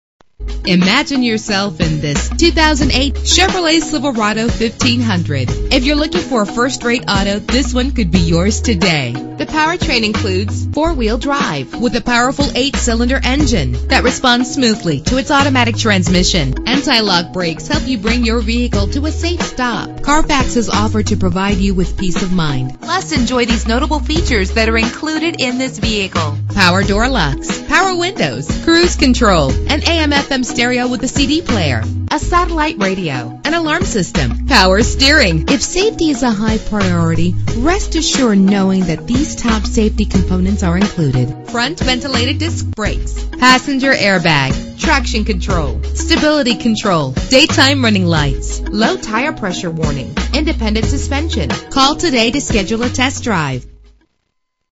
2008 Chevrolet Silverado 1500 - Clements Chevrolet Cadillac Subaru
Year: 2008
Make: Chevrolet
Model: Silverado 1500
Trim: LT
Engine: 5.3 liter 8 cylinder 16 valve
Transmission: 4-Speed Automatic
Color: Greystone Metallic
Mileage: 38881
Address: 1000 12th st. SW